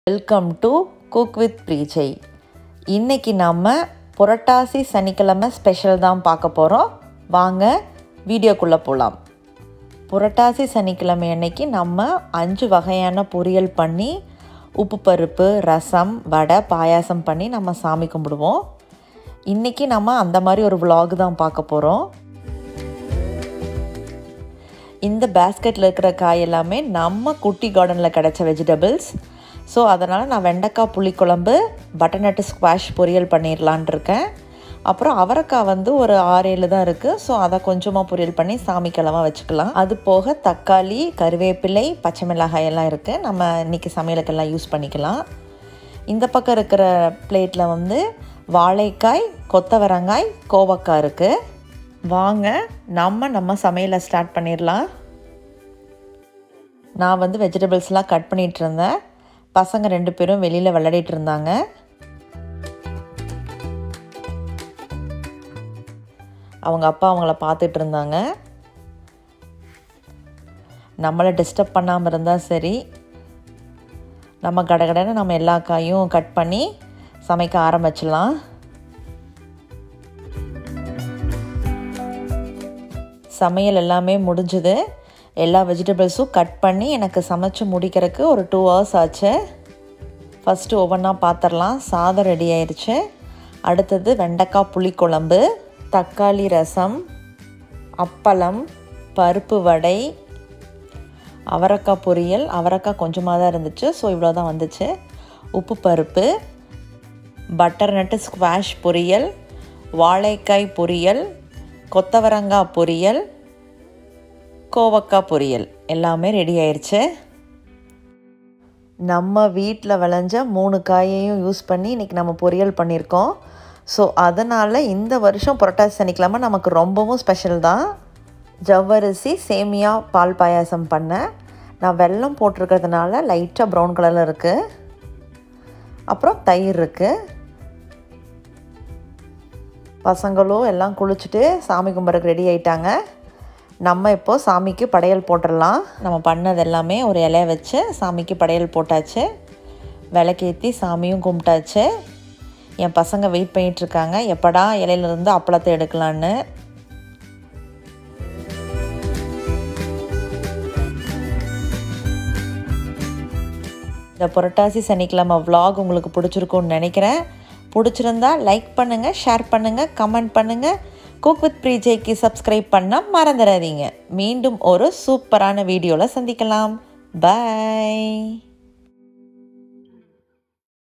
Purattasi Special Lunch with Home Grown Veggies | Purattasi Special Tamil Vlog | USA |CookwithPrijay
In this video Purattasi Special Lunch with Home Grown Veggies.

1. Dal/Uppuparuppu
2. Ladies Finger Pulikulambu (Home Grown Veggie)
3. Butternut Squash Poriyal (Home Grown Veggie)
4. Avarakkai Poriyal (Home Grown Veggie)
5. Valaikkai Poriyal
6. Cluster Beans Poriyal
7.Kovakkai Poriyal
8. Rasam
9. Paruppu Vadai
10. Javvarisi Semiya Payasam
11. Apparam
12.Curd
13. Rice